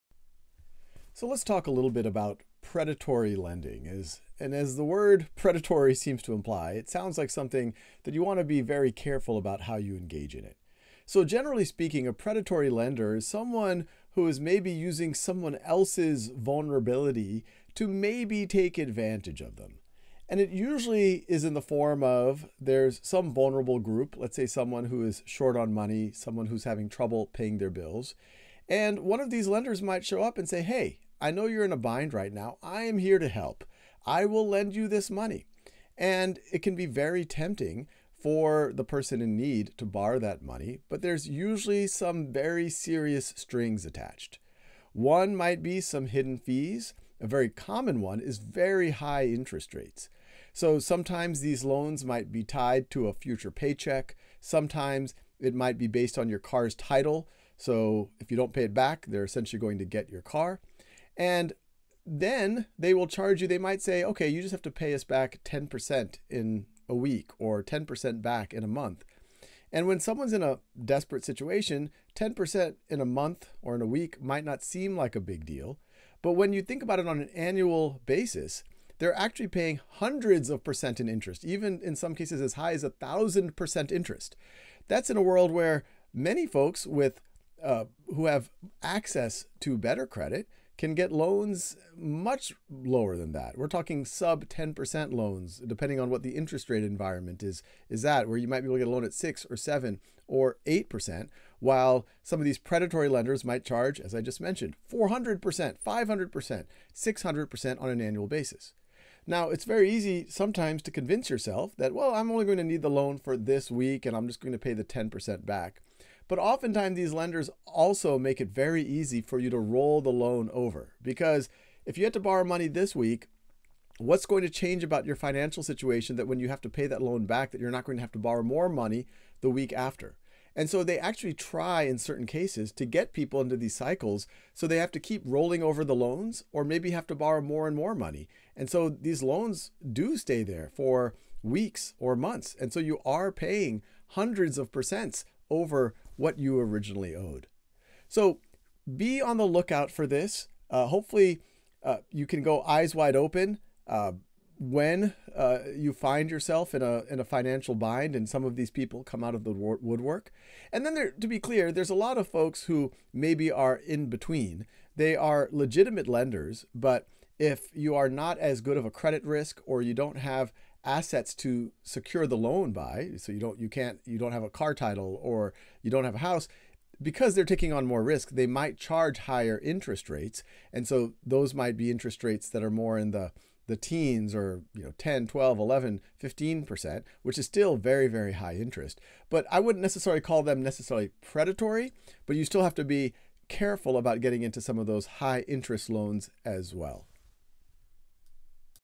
Predatory lending | Loans and debt | Financial Literacy | Khan Academy
Predatory lending is when lenders use unfair tactics like very high-interest rates or hidden costs to take advantage of borrowers, especially those who really need a loan and have few other options. This can trap people in a cycle of debt, making it very hard for them to get out.